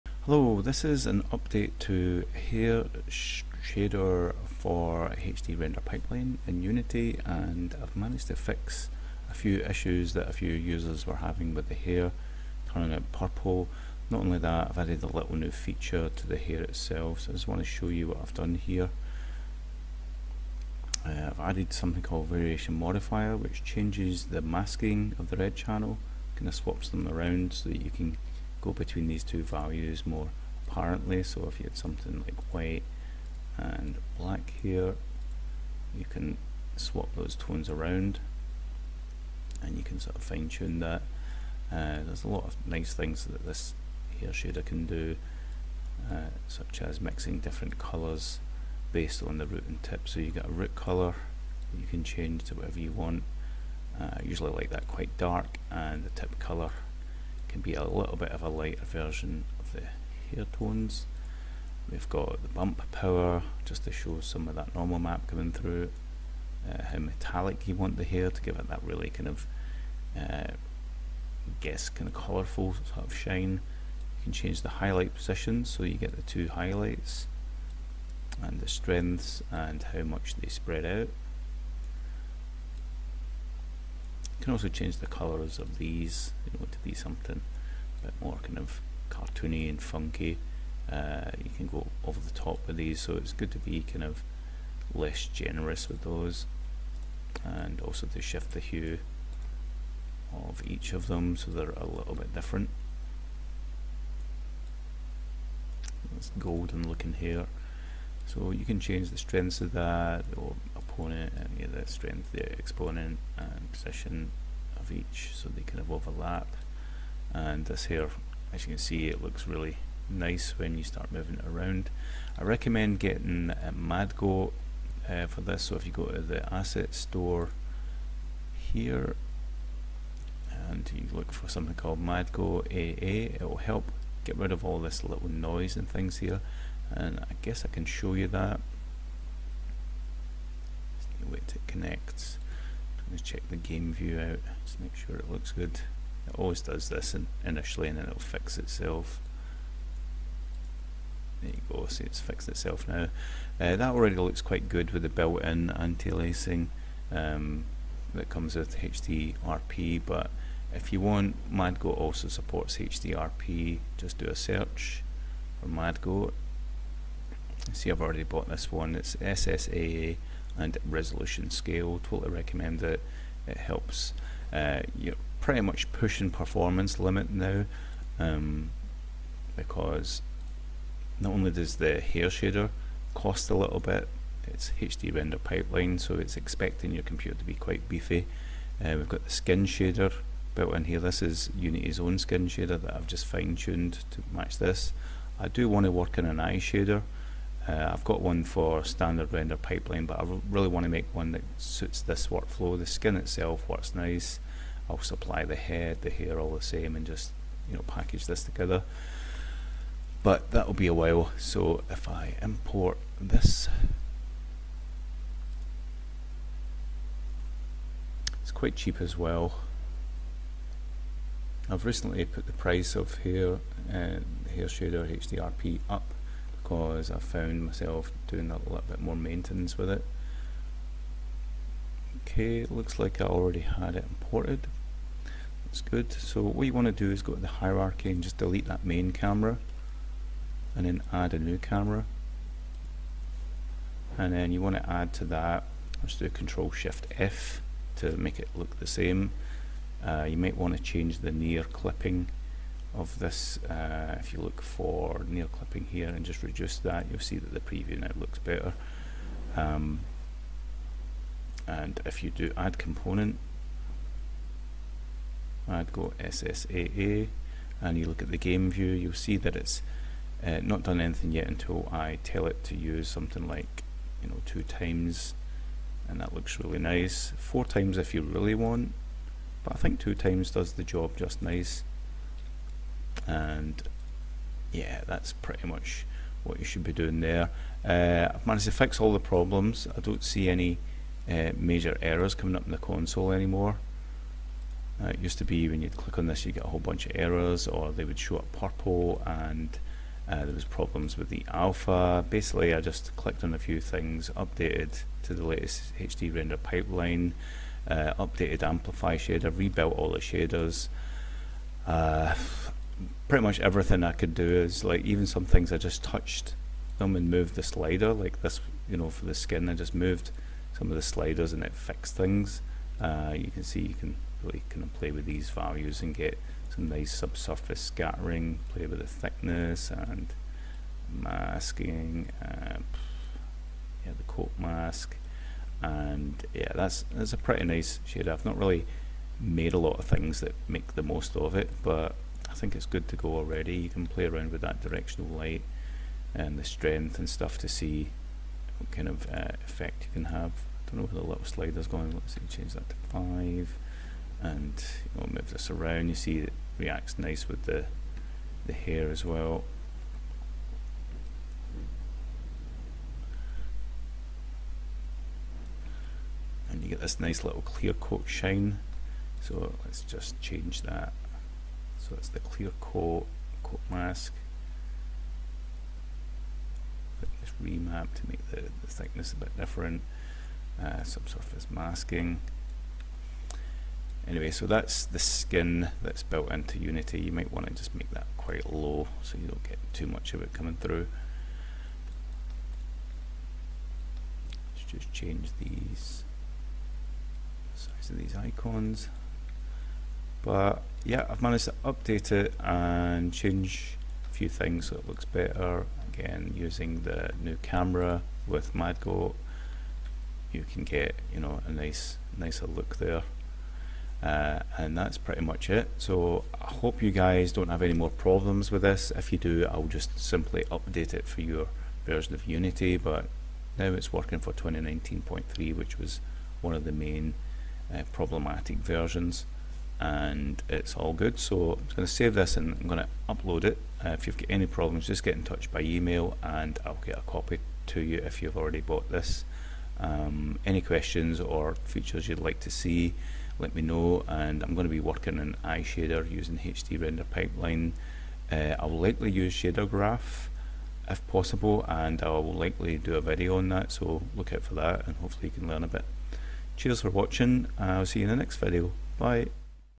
Hair Shader HDRP - Unity 2019.3 Fixed and Updated
Finally got a decent fix to Hair Shader - HDRP for Unity. Also a little run down on some features. If you have any issues with the shaders not showing correctly, be sure to update your RP Core and High Definition packages to the latest preview. Bear in mind HDRP is still in development and I am doing my best to keep up with it's changes and all the iterations of Unity.